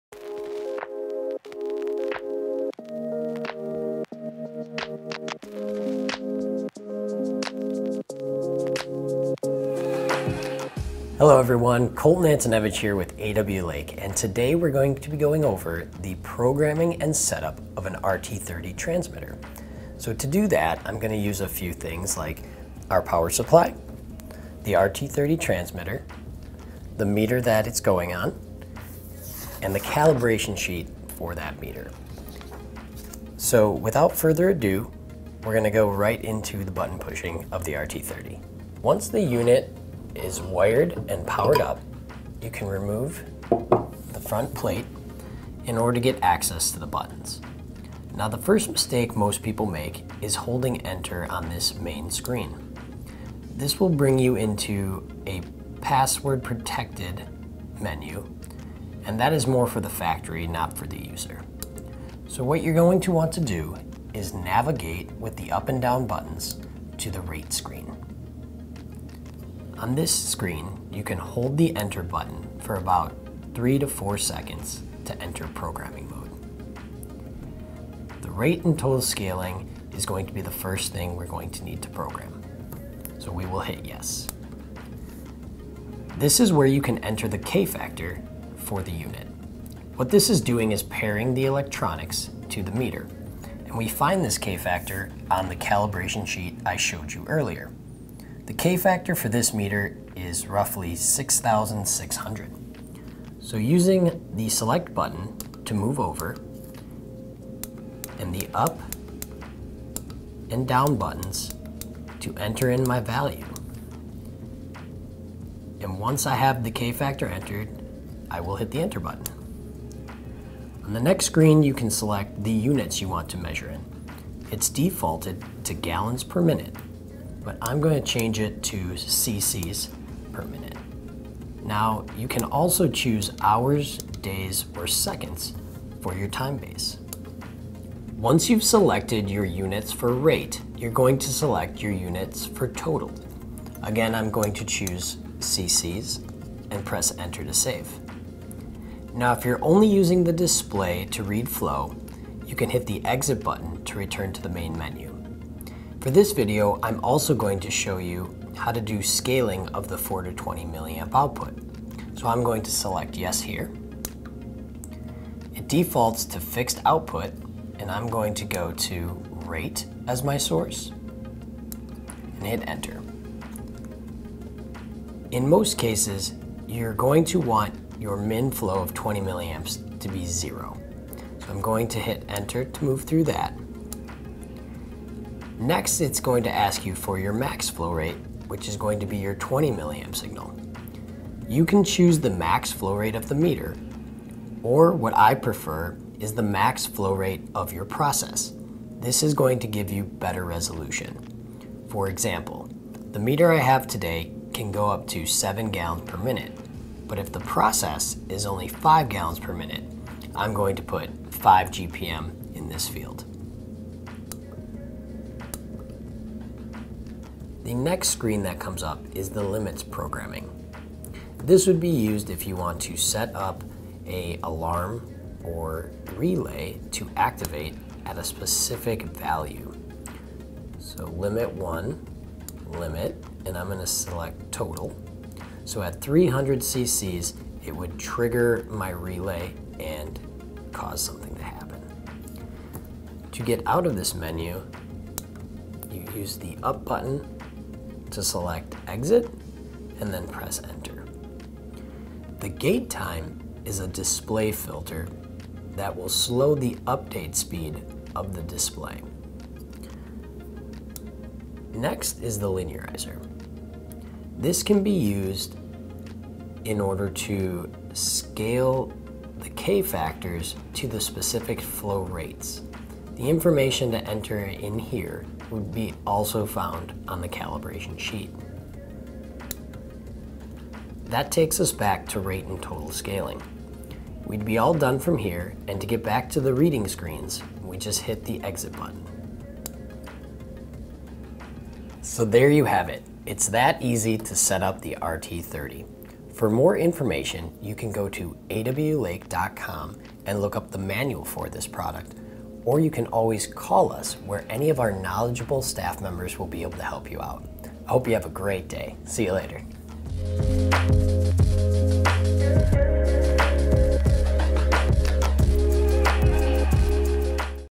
AW-Lake FAQ: How Do I Program a RT-30 Flow Rate Transmitter
We here at AW-Lake receive many questions regarding how to program the RT-30 Flow Rate Transmitter. In this video Coltan will walk you through how to setup the RT-30.

If you have any questions please contact our team at AW-Lake.com.

1:25 - Programming K-Factor

2:01 - Programming different units of rate measurement

2:18 - Programming different units for totals

2:35 - Programming scaling

3:38 - Programming limits

4:12 - Programming gate times

4:21 - Programming the linearization